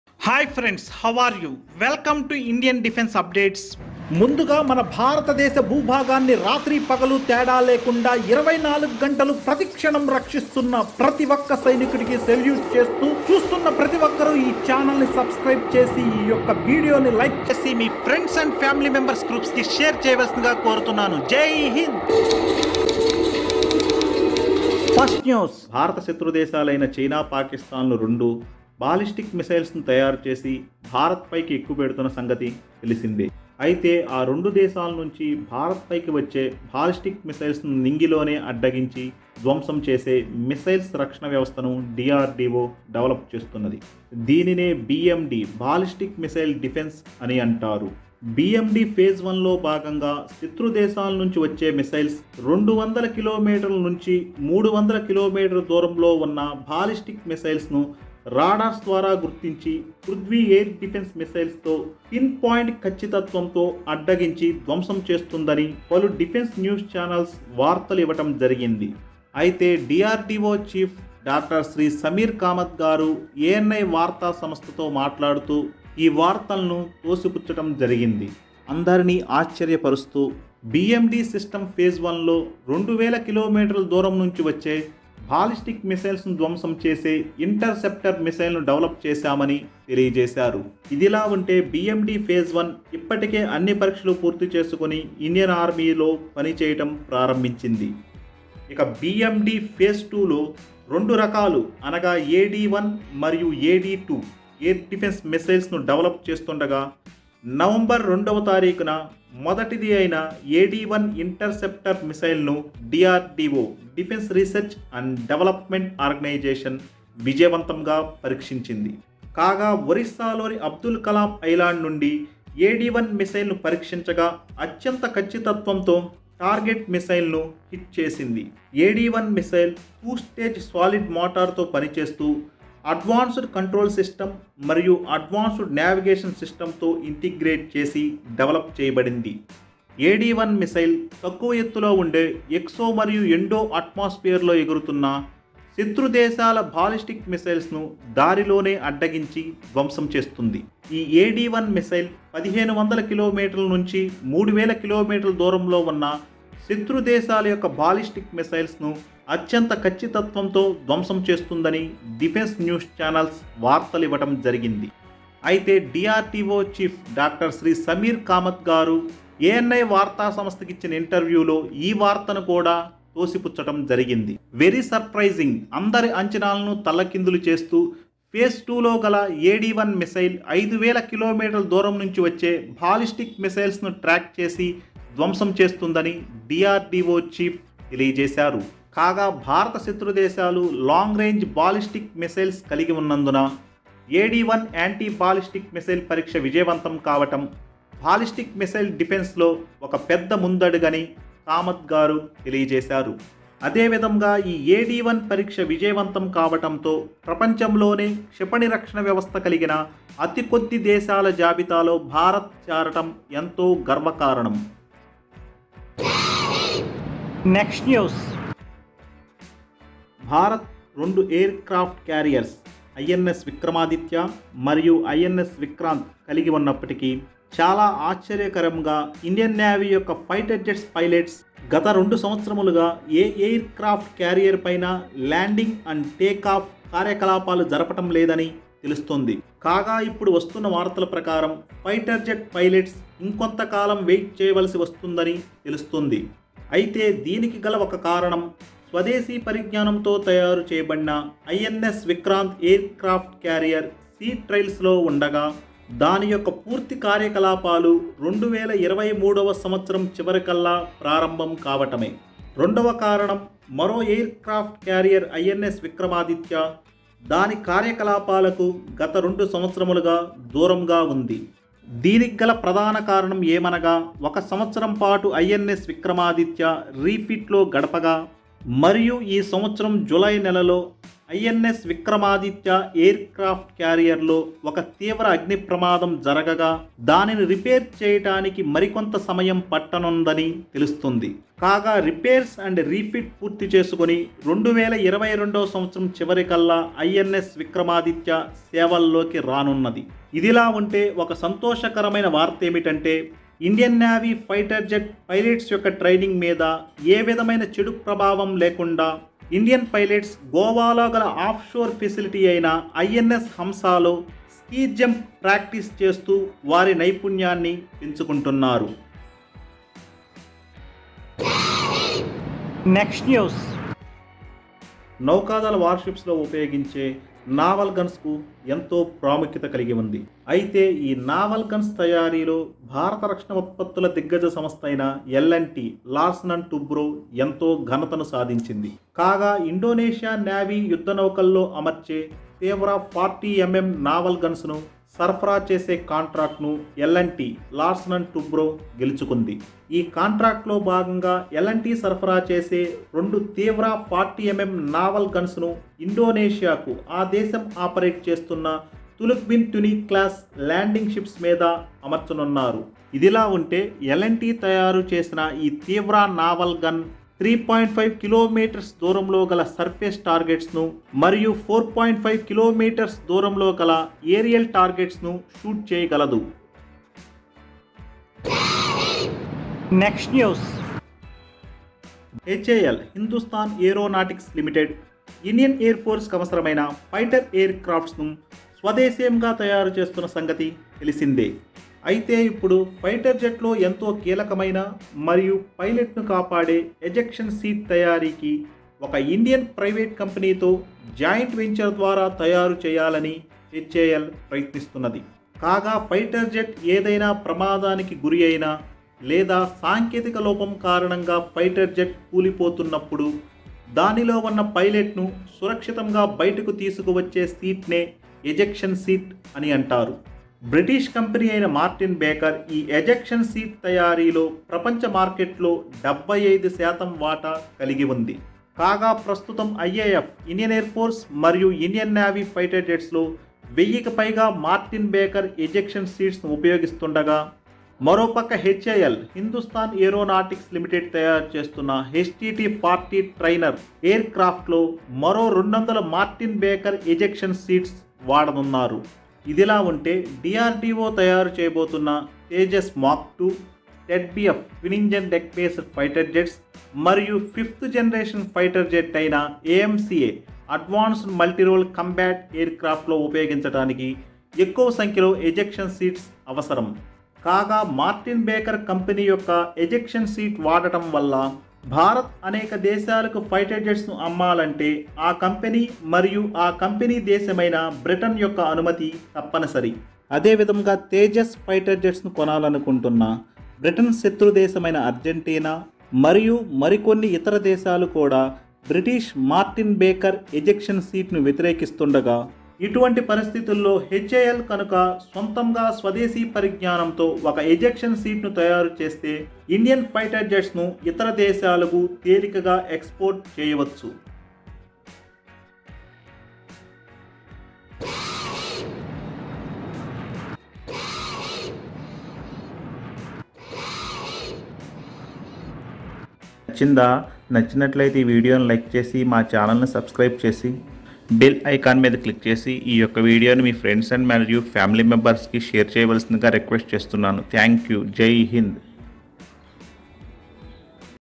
INDIAN DEFENCE NEWS AND UPDATES TELUGU AD-1
indian DEFENCE NEWS AND UPDATES TELUGU
L&T NAVAL GUN EXPORT TO INDONASIA
EJECTION SEAT BY HAL
insvikrant insvikramaditya FIGHTER JET TAKE OFF AND LANDING
TELUGUDEFENCE NEWS